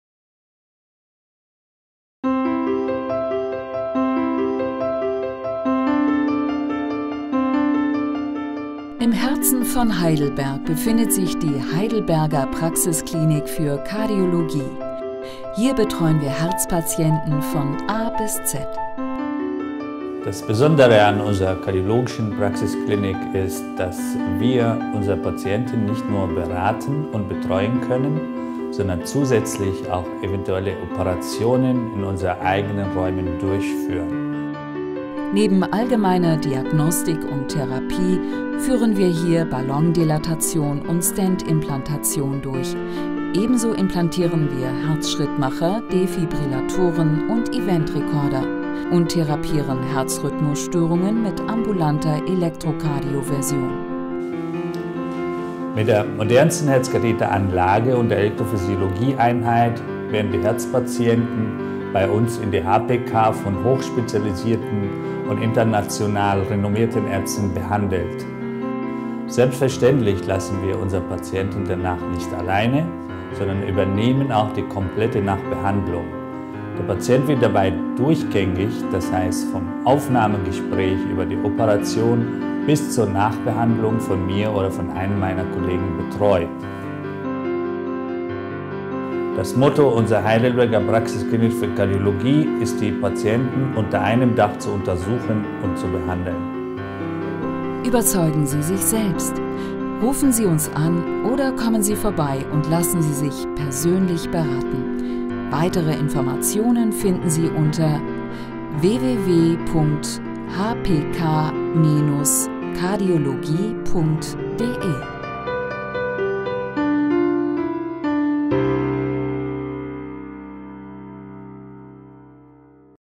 Die Heidelberger Praxisklinik für Innere Medizin, Kardiologie & Rhythmologie (HPK) stellt sich vor
Damit Sie sich ein Bild von der HPK machen können, stellen wir unsere Praxisklinik und ganz besonders unser hochmodernes Herzkatheterlabor in diesem Film vor. Wenn Sie Fragen zum Thema Kardiologie oder unserer Praxisklinik haben, Besuchen Sie unsere